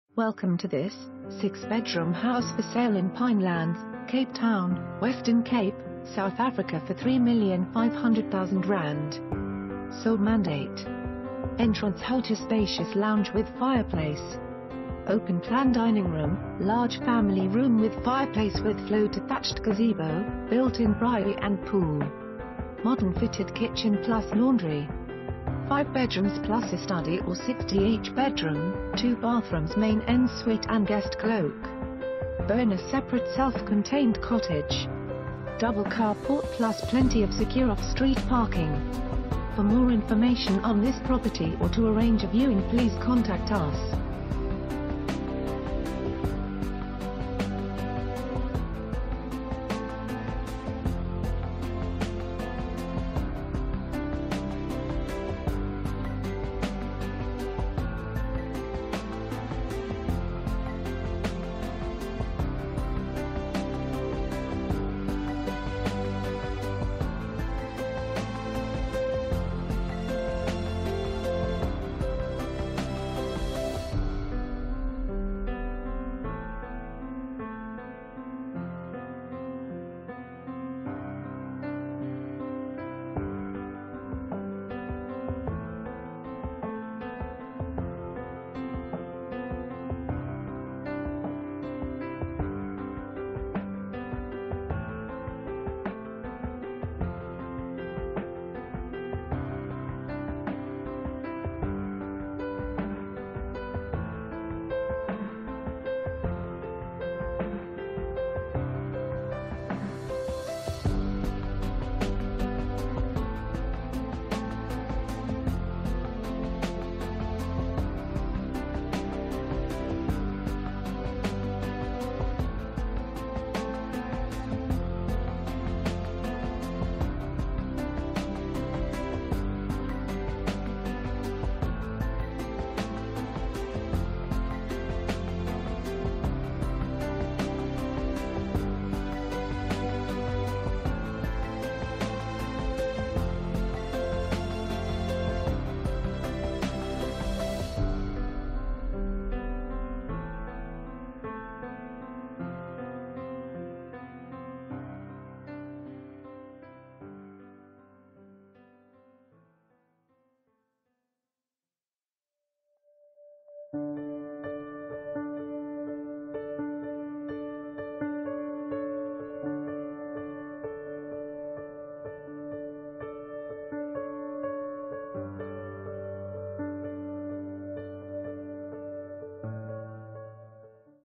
6 Bedroom House For Sale in Pinelands, Cape Town, Western Cape, South Africa for ZAR 3,500,000
Sole Mandate.
Entrance hall to spacious lounge with fireplace.
Open plan dining room, large family room with fireplace with flow to thatched gazebo, built in braai and pool. Modern fitted kitchen plus laundry.
5 Bedrooms plus a study or 6th bedroom, 2 bathrooms (main en suite) and guest cloak.
BONUS: Separate self contained...